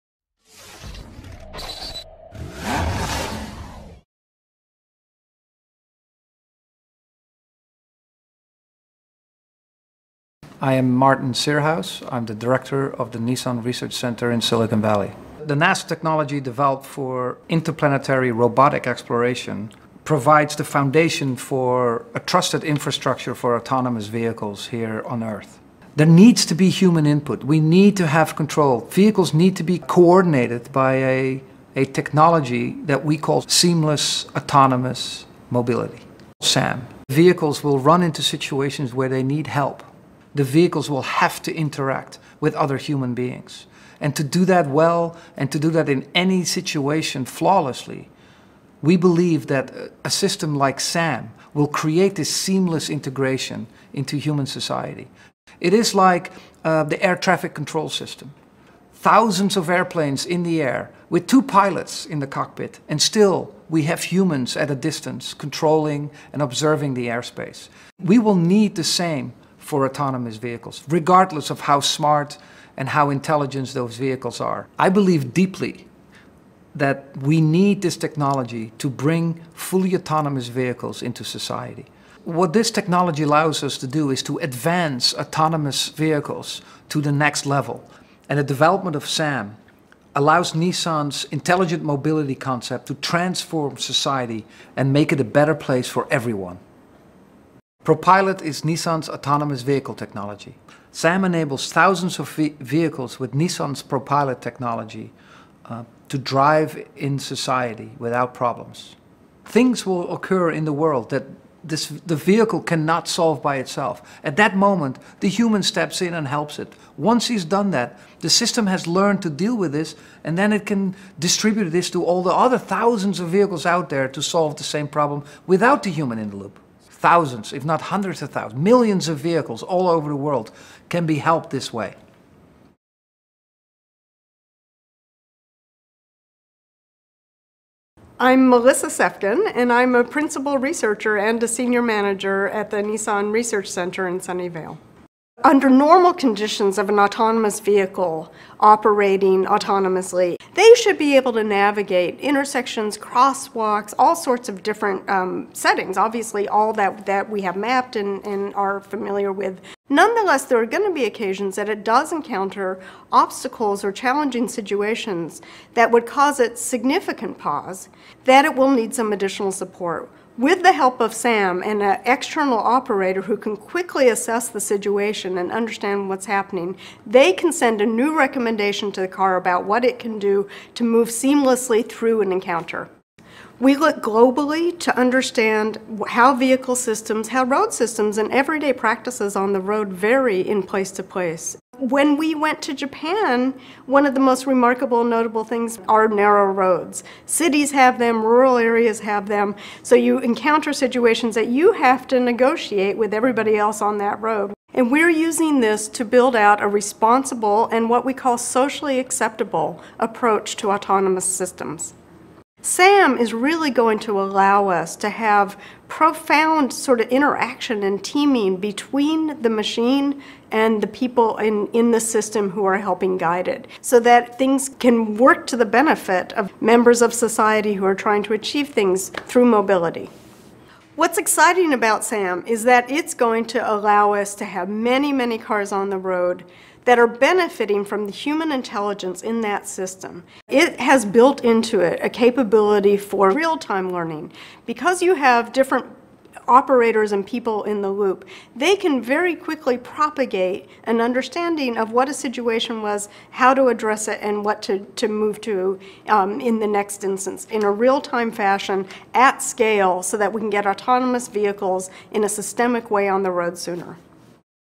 Nissan Seamless Autonomous Mobility (SAM)
To accelerate the time it will take for autonomous vehicles to get on the road safely, at CES 2017 Carlos Ghosn announced a breakthrough technology called "Seamless Autonomous Mobility," or SAM. Developed from NASA technology, SAM partners in-vehicle artificial intelligence (AI) with human support to help autonomous vehicles make decisions in unpredictable situations and build the knowledge of in-vehicle AI. This technology could potentially enable millions of driverless cars to co-exist with human drivers in an accelerated timeline. It is part of Nissan Intelligent Integration.